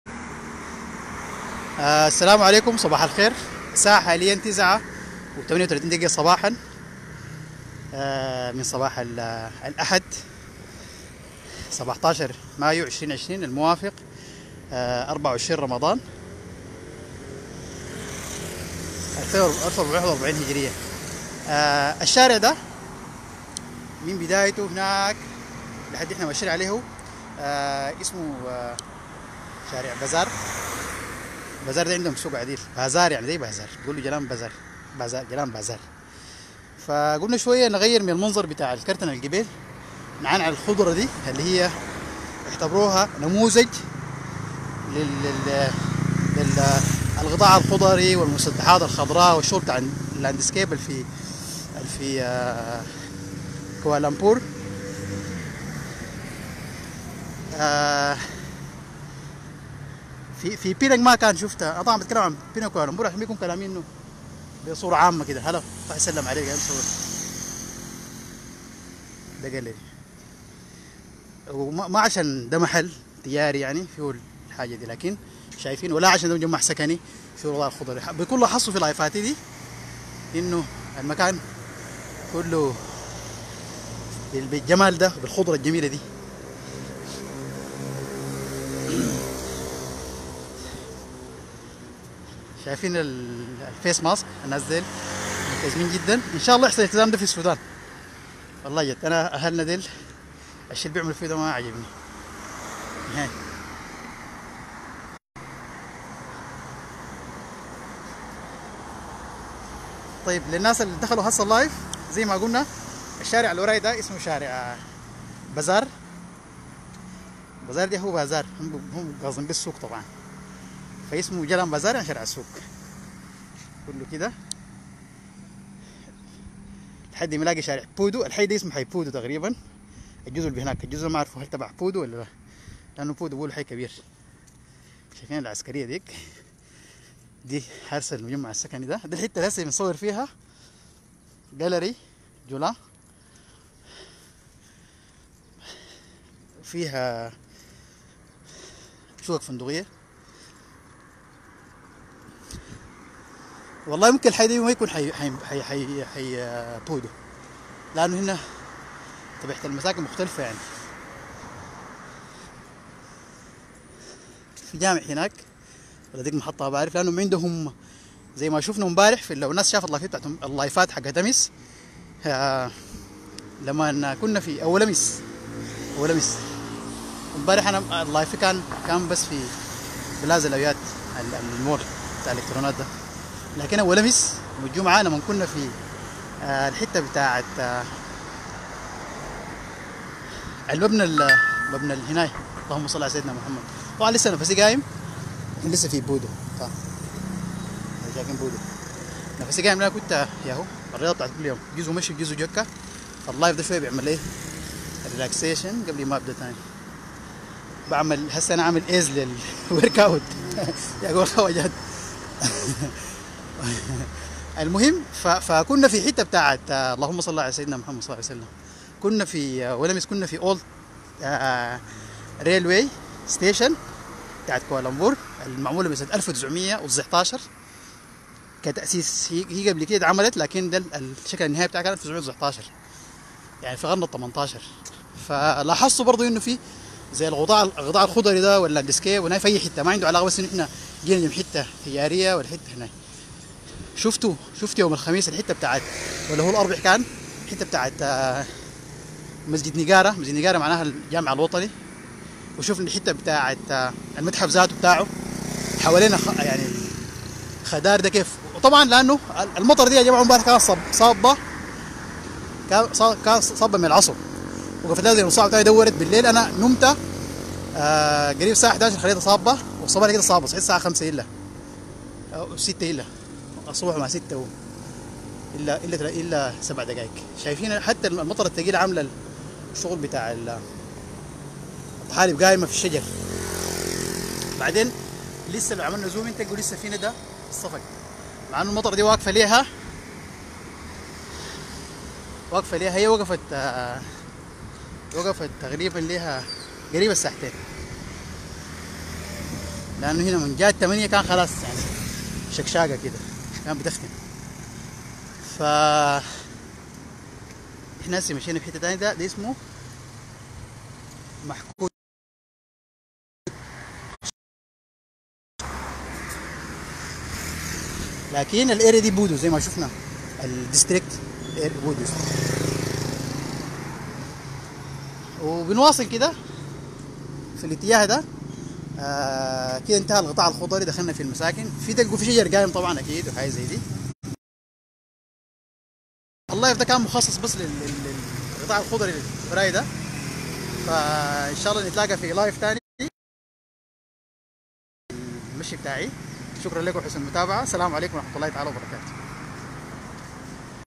Landscape design in Kuala Lumpur : Jana Pasar as random choose
Landscape design in Kuala Lumpur :  Jalan Pasar as random choose

A Facebook's live by the Sudanese writer MuzamilAlbagir مزمل_الباقر meditated in the landscape design of Pasar Street in Pudu District

كوالالمبور ماليزيا
malaysia